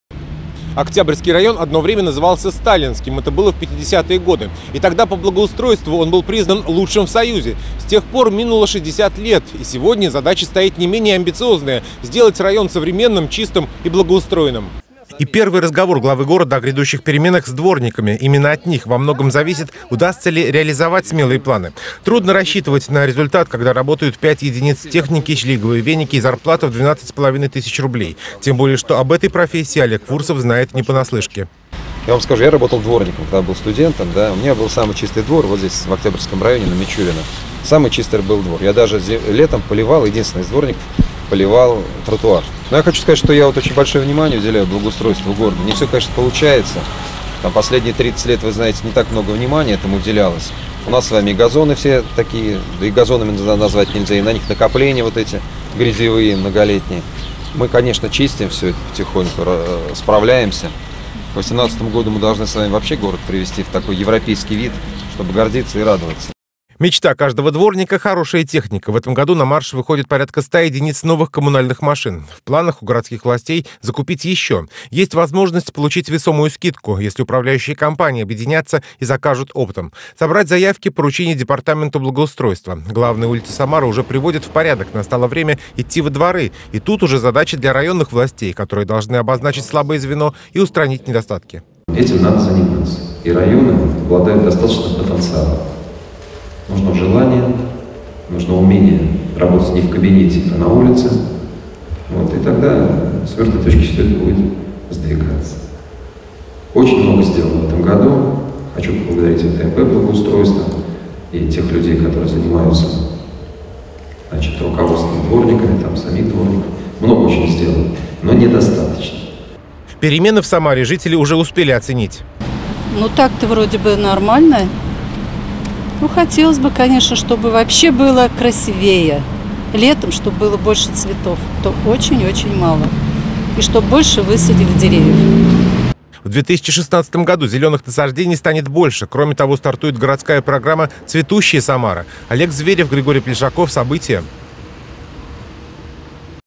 Масштабное преображение Самары начинается с Октябрьского района
Все силы - на благоустройство. Преображение Самары становится похожим на стройки первых пятилеток. Впервые за тридцать лет задача навести порядок в рекордные сроки обозначена как стратегически важная.  Свой рабочий день Олег Фурсов начал с поездки по Октябрьскому району.